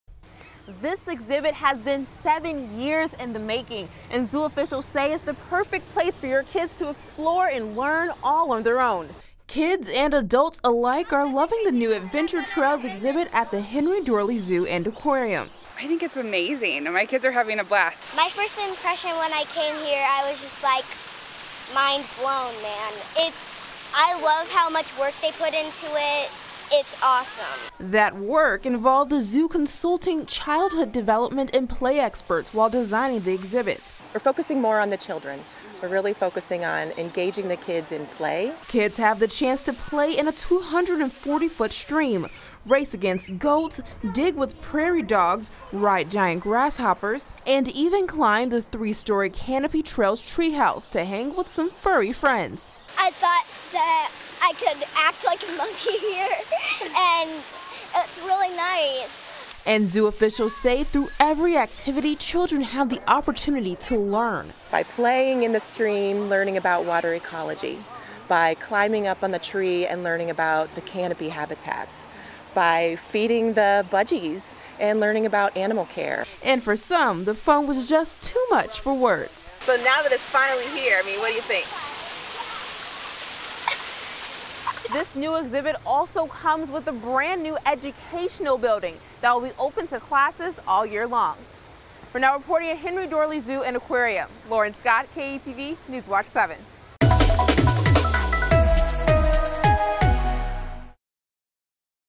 New kid-focused interactive exhibit opens at Henry Doorly Zoo
Kids from across the area check out the Henry Doorly Zoo's new Children's Adventure Trails exhibit.